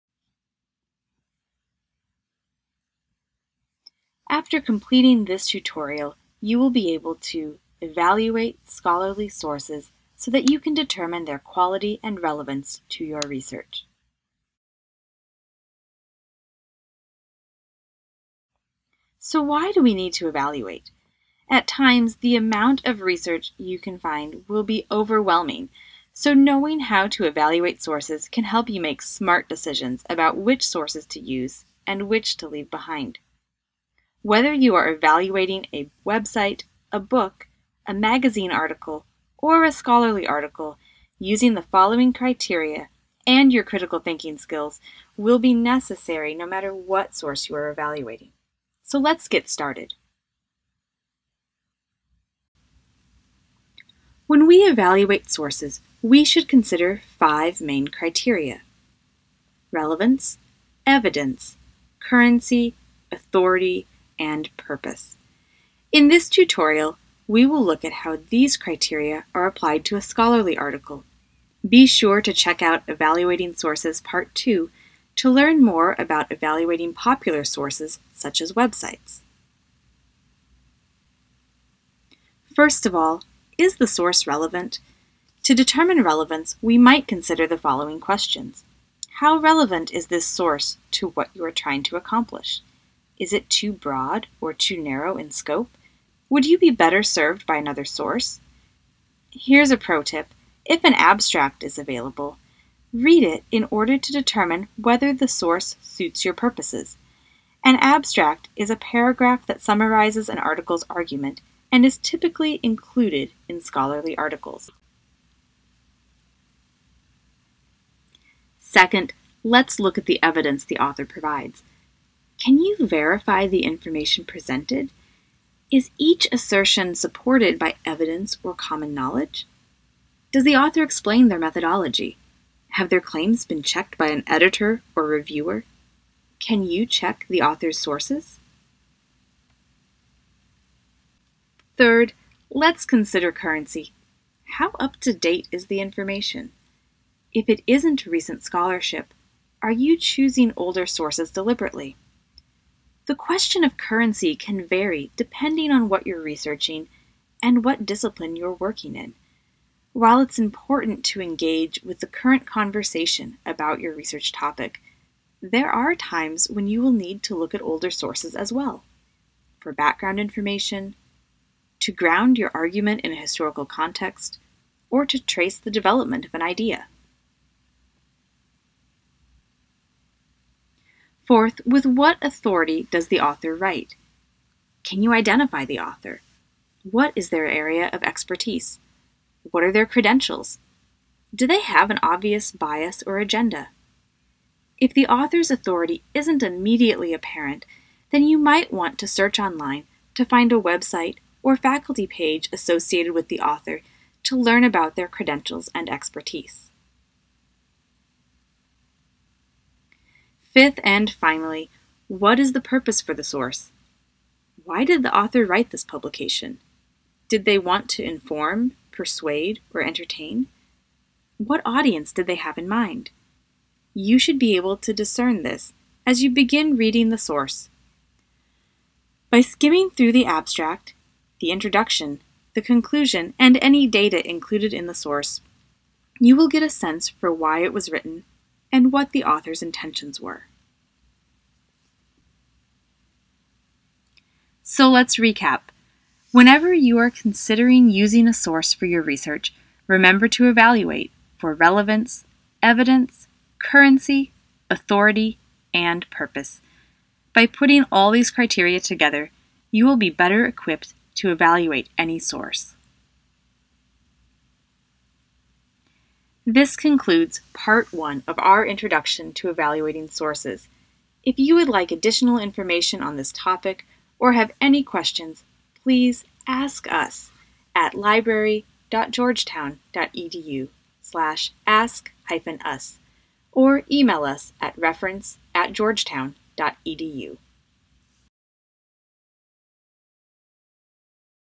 Evaluating Sources, Part 1: Scholarly Sources
This tutorial teaches how to evaluate scholarly articles based on relevance, evidence, currency, authority, and purpose.